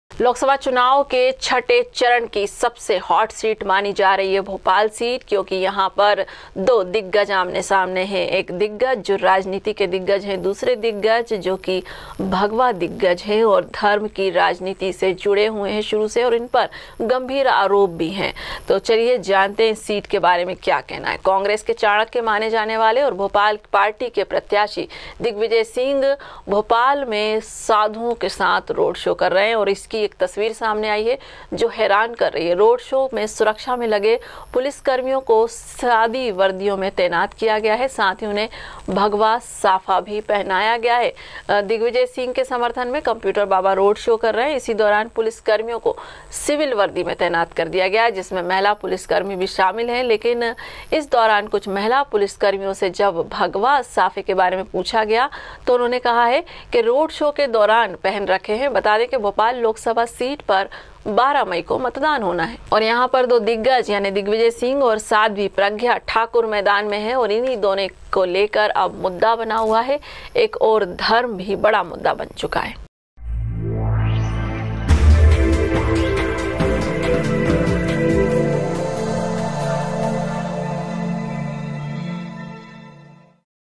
भगवा दिखे दिग्विजय का रोड शो, साफा पहनाकर भीड़ में शामिल किए गए पुलिसकर्मी / THE NEWS INDIA
लोकसभा चुनाव के छठे चरण की सबसे हॉट सीट माने जाने वाली भोपाल पर दंगल शुरू हो गया है. कांग्रेस के चाणक्य माने जाने वाले और भोपाल से पार्टी के प्रत्याशी दिग्विजय सिंह आज भोपाल में साधुओं को साथ लेकर रोड शो कर रहे हैं. लेकिन इसी रोड शो एक ऐसी तस्वीर सामने आई है, जो हैरान करती है. रोड शो की सुरक्षा में लगे पुलिसकर्मियों को सिविल वर्दी में तैनात किया गया है